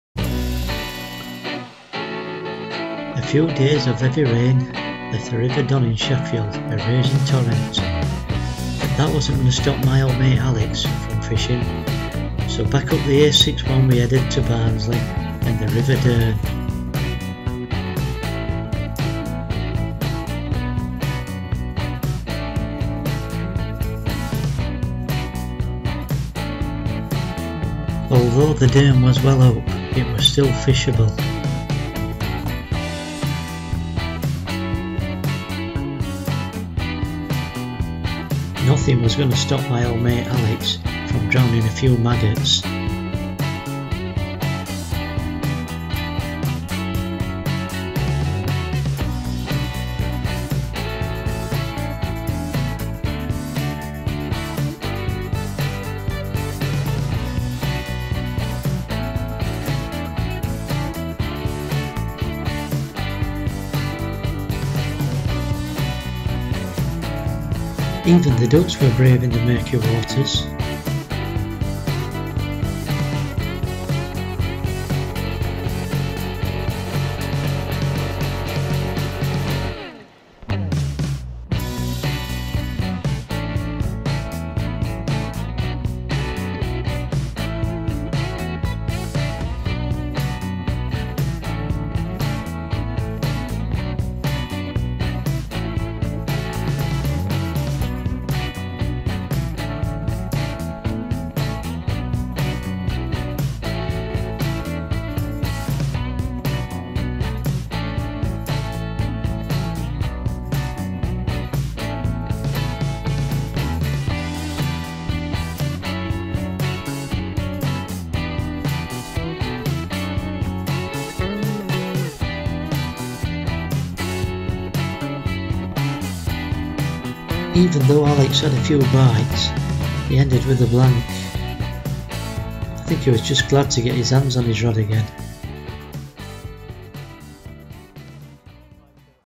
Trailer
A few days heavy rainfall left a planned fishing session on the River Don in Sheffield a raging torrent and unfishable. But that wasn't going to stop my old mate Alex Dalton a fishing fanatic from drowning a few maggots so back up the A61 he went to Barnsley and a quick session on the River Dearne.